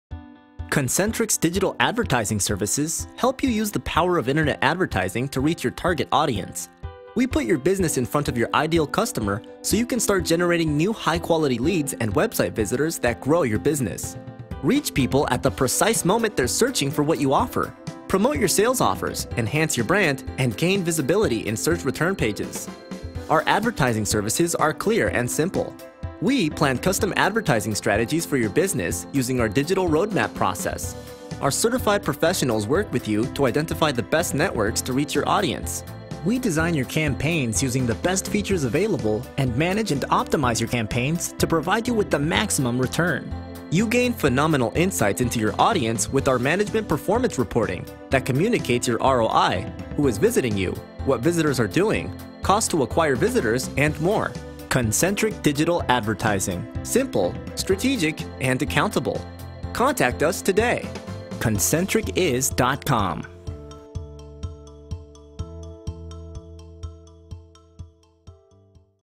Concentric Digital Advertising Services
Whiteboard Animation that describes Concentric Digital Advertising Services including AdWords, Facebook, Instagram, PPC and Remarketing Advertising.